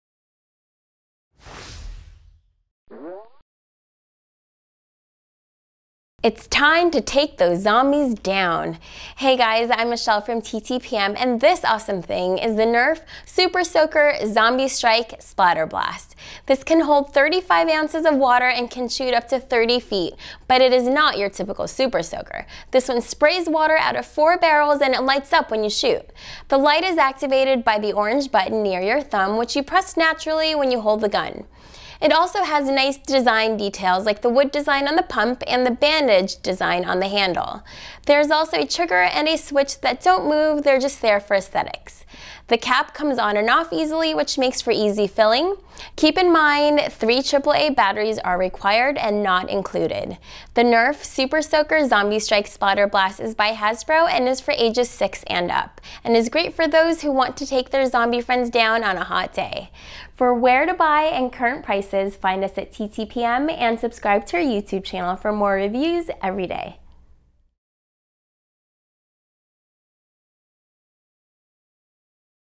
Nerf Super Soaker Zombie Strike Splatterblast from Hasbro
The Nerf Super Soaker Zombie Strike Splatterblast Blaster from Hasbro is a water blaster that sprays out of four barrels and lights up. For full review and shopping info

Product Info:
The light is activated by the orange button near your thumb, which you press naturally when you hold the gun. This can hold 35 ounces of water and can shoot up to 30 feet. It has nice design details like the wood design on the pump and the bandage design on the handle. The cap comes on and off easily, which makes for easy filling. You'll go through water pretty fast with this, but that's the whole point.